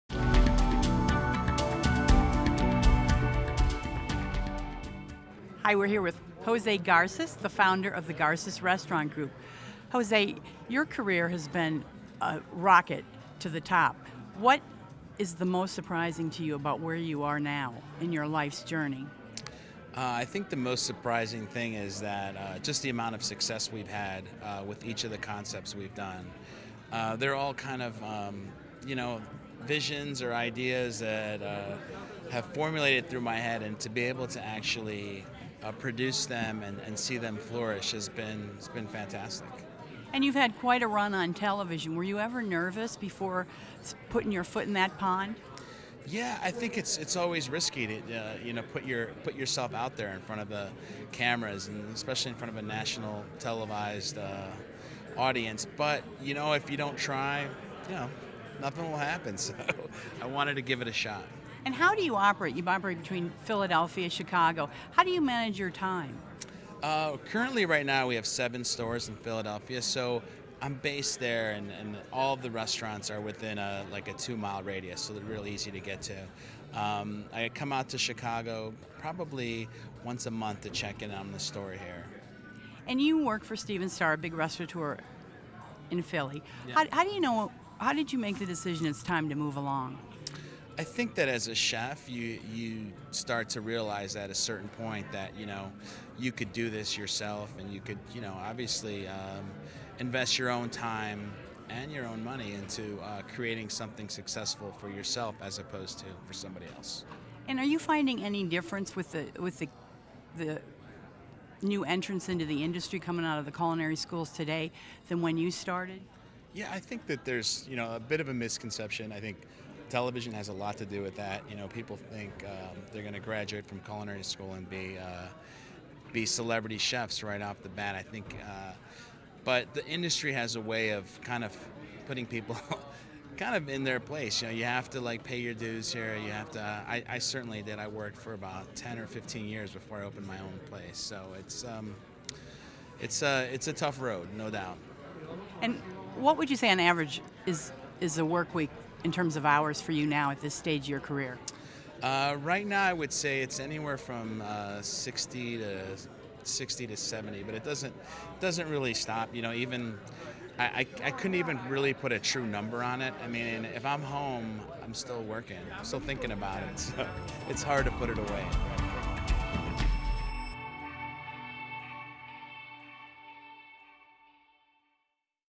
PRO Chat: Jose Garces
Jose Garces is the owner and executive chef of the Garces Restaurant Group. Since 2005 Garces has opened six restaurants in Philadelphia and Chicago including Amada, Tinto, Distrito, Chifa, Village Whiskey and Mercat a la Planxa. Additionally Garces authored Latin Evolution and is one of just a handful of chefs holding the title of Iron Chef America.